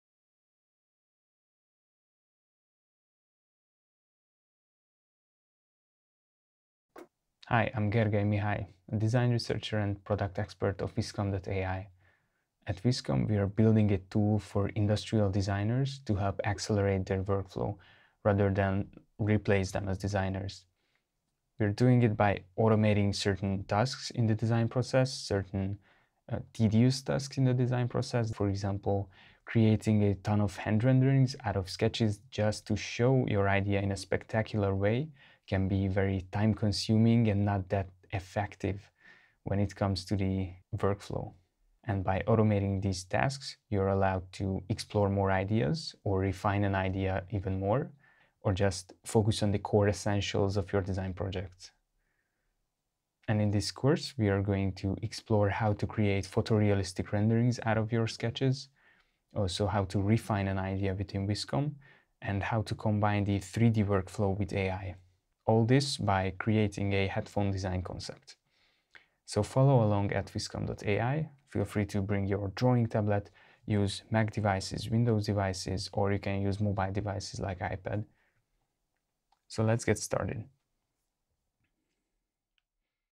Introduction to Vizcom - Unleashing Your Creative Potential - Part 1
In this introductory tutorial, embark on a journey to unlock your creative potential with Vizcom. Discover the intuitive interface, powerful features, and endless possibilities that await you in this groundbreaking design tool.